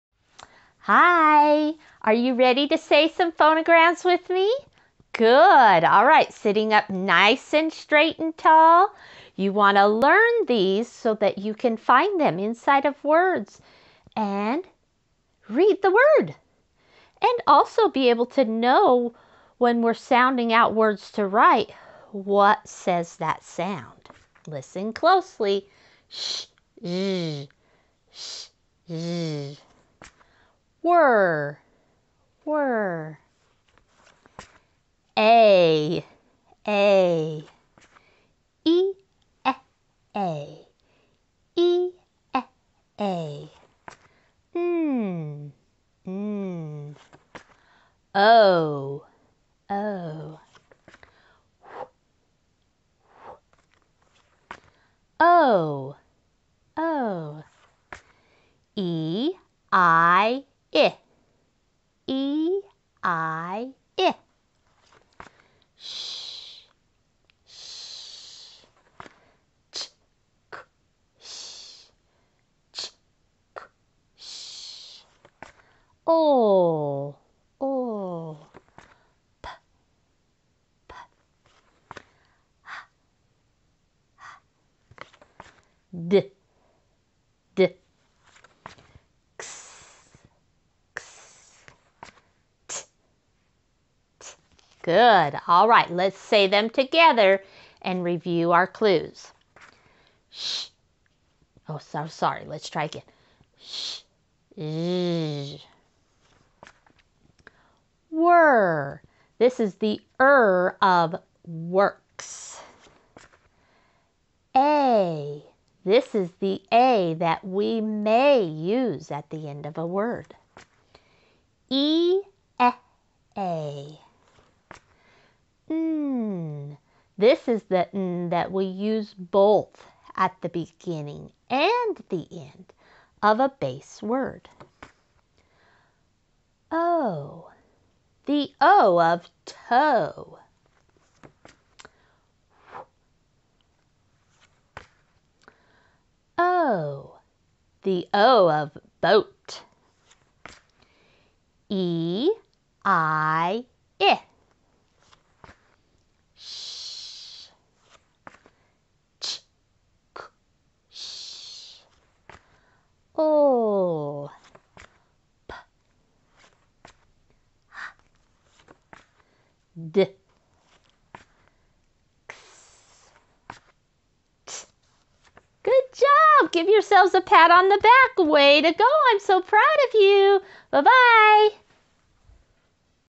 Set 2 of Phonograms - Learn to read and write
Say these phonograms everyday with me.  Learn the sounds of the letters and letter combinations; so you can sound out words while reading.  Learn what letter or letter combinations say a particular sound; so you can spell words correctly.